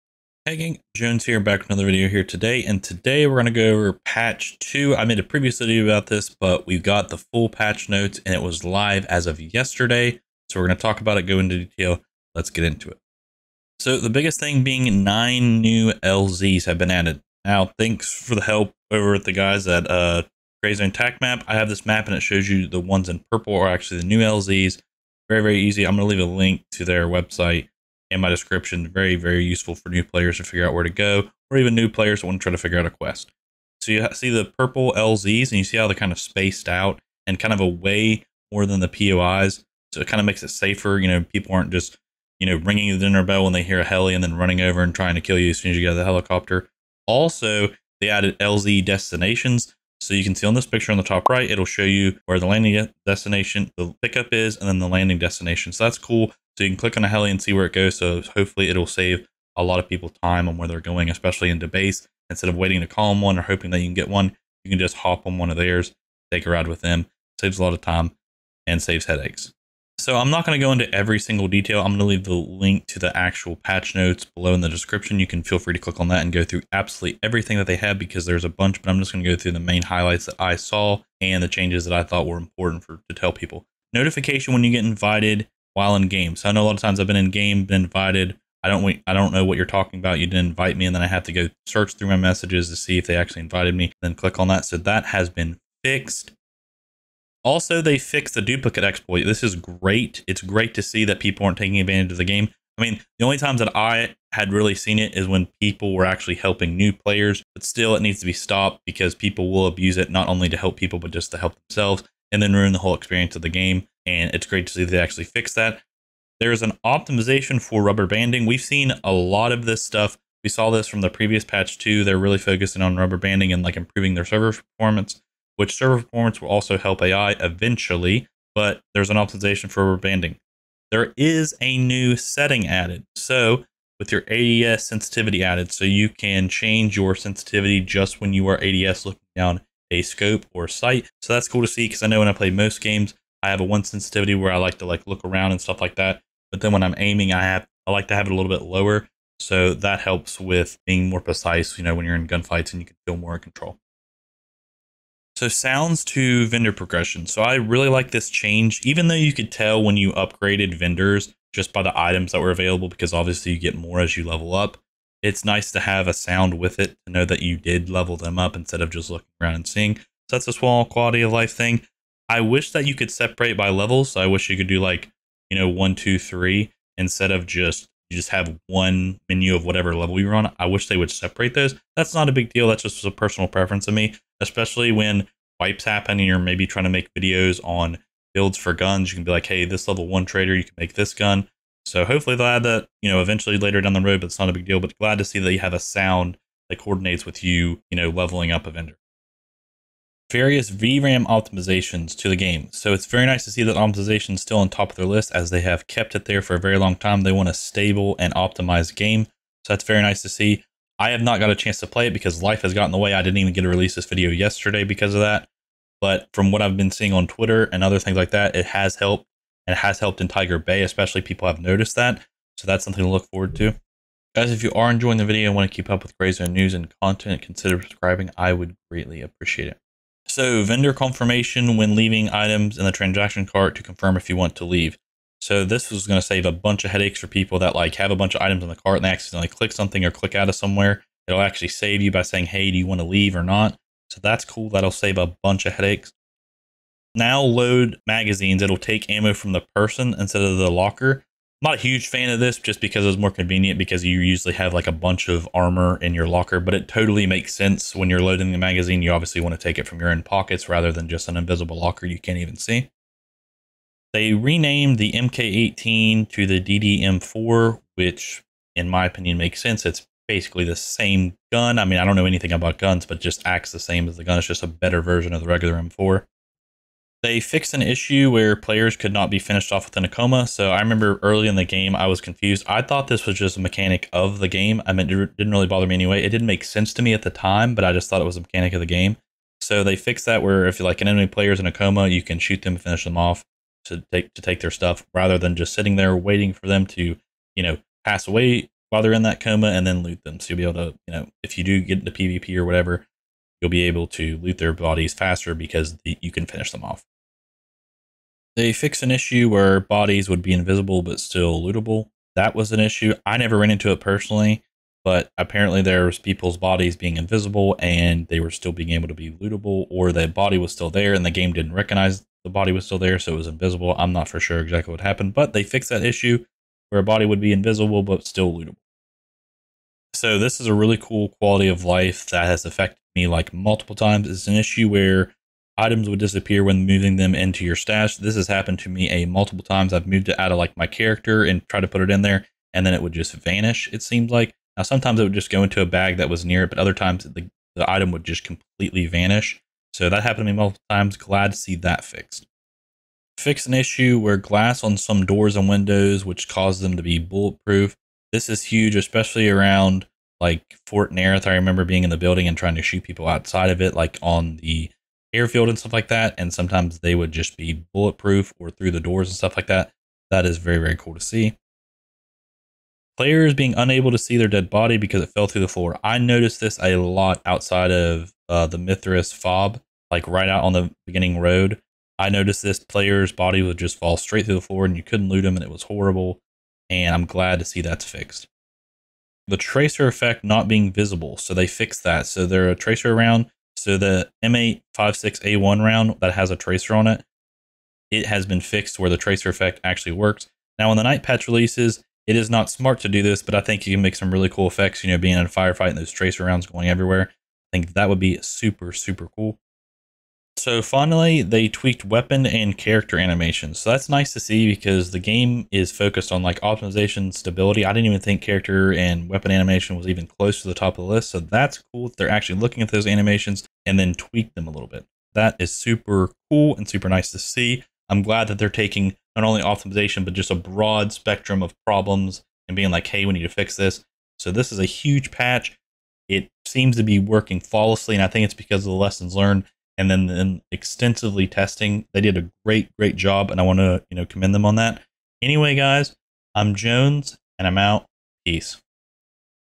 Gray Zone Warfare Patch 2 Details!
Hi gang we talk about Patch 2 just released by Gray Zone Warfare! We go into the big details of the patch and talk about them! Hope you enjoy!

Chapters

Intro - 0:00
Key New Features - 0:12
Fixes, Optimizations, and Small Additions - 1:15